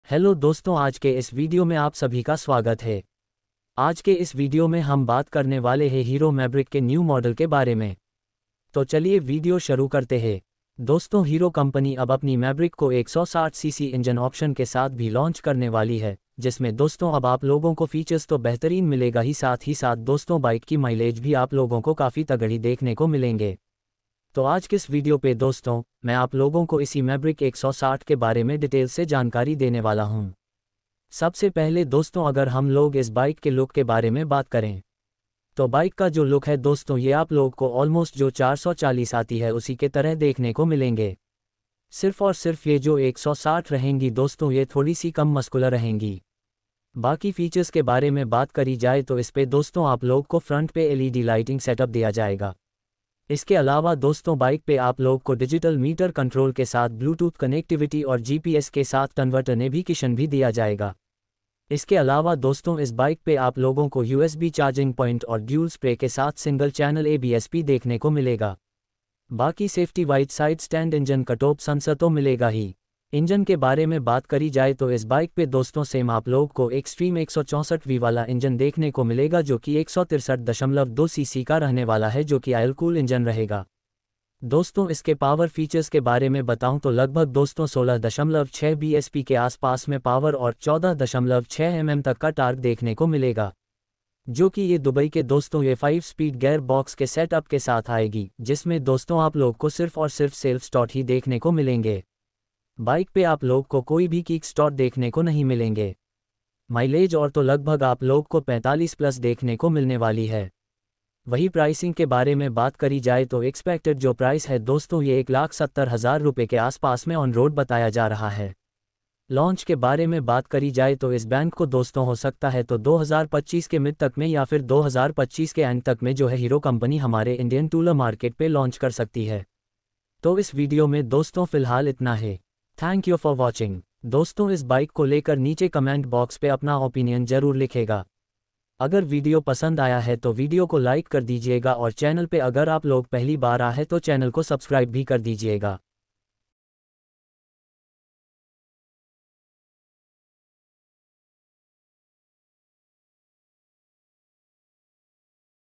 Hero Mavrick 160CC Review - Price, design,features, performance | Hero Mavrick 160cc launch confirm
— Key Specs & Features of Hero Mavrick 440

- Engine : 440 cc
- Power : 27.36 PS
- Torque : 36 Nm
- Kerb Weight : 191 kg
- Brakes : Double Disc
- Tyre Type : Tubeless

About Hero Mavrick 440

Hero Mavrick 440 is a motorcycle with a starting price of Rs 2 Lakh. It is available in India in 1 variant and 5 colours with high end variant price starting from Rs 2 Lakh. Mavrick 440 is powered by a 440 ccbs6-2.0 engine which develops a power of 27.36 PS and a torque of 36 Nm. It has Disc front brakes and Disc rear brakes. The weight of Hero Mavrick 440 is 191 kg and comes with a fuel tank capacity of 13.5 L.

The Hero Mavrick has been unveiled at Hero World 2024 and now we have a clearer picture of the motorcycle’s engine, specifications and features, read all about it here.

It will be powered by the 440cc engine that we’ve seen on the Harley-Davidson X440, making 27.37PS and 38Nm. The bike is equipped with a round headlight with LED indicators. It has sporty tank extensions. The Mavrick gets a roomy single-piece seat and a single-piece GRabrail,
At the rear, the tail section has a longer licence plate holder than the Harley X440. Underpinnings include a telescopic fork and twin shocks at the rear. The Mavrick is equipped with machined alloy wheels in the top-end variant and spoke wheels in the base variant.

As for the console, it too seems to be different from the X440’s centrally mounted TFT one. The Mavrick 440 gets a negative  LCD instrument console with speedometer, tachometer (with the same 6000rpm redline as the X440), gear position indicator, remaining range and mileage indicator, and side stand alert. Additionally, the bike also gets bluetooth connectivity with turn-by-turn navigation, call and SMS alert, a digital clock, Estimated Time Of Arrival (ETA), and distance, and a phone battery indicator. It also gets e-Sim connectivity that allows access to remote tracking among 35 other features.